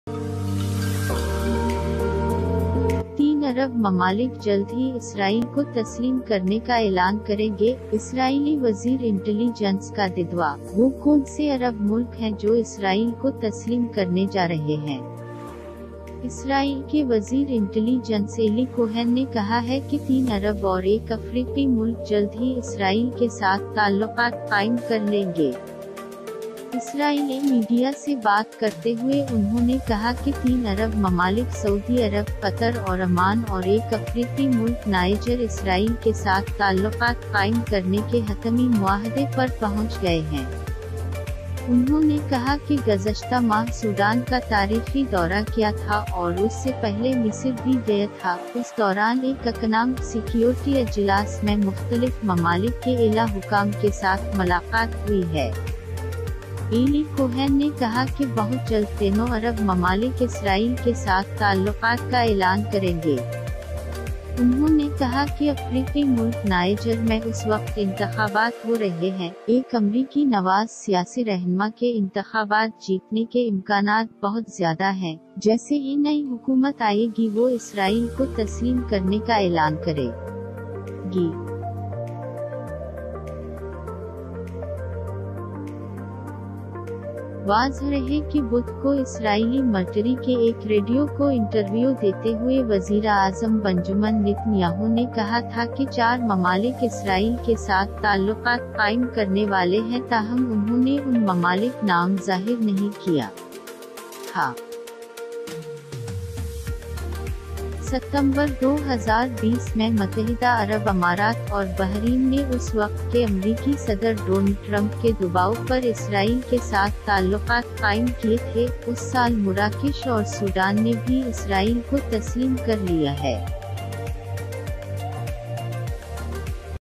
Three Arab countries will soon announce their recognition of Israel!
Israeli intelligence minister claims!
 What Arab countries are going to recognize Israel!
 Israeli Intelligence Minister Eli Cohen has said that three Arab and one African country will soon establish relations with Israel!

 Speaking to Israeli media, he said that three Arab countries, Saudi Arabia, Qatar and Oman, and an African country, Niger, have reached a final agreement to establish relations with Israel.

 He said that he had paid a historic visit to Sudan last month and before that he had also visited Egypt.

 Eli Cohen said that soon the three Arab countries will announce relations with Israel!

 "Elections are taking place in the African country of Niger at the moment! The chances of a pro-American political leader winning the election are very high! As soon as the new government comes, it will announce its recognition of Israel!"

 It should be noted that in an interview with an Israeli military radio station on Wednesday, Prime Minister Benjamin Netanyahu had said that four countries are going to establish relations with Israel, but he did not reveal the names of these countries!

 In September 2020, the United Arab Emirates and Bahrain established relations with Israel under pressure from then-US President Donald Trump.  This year, Morocco and Sudan have also recognized Israel!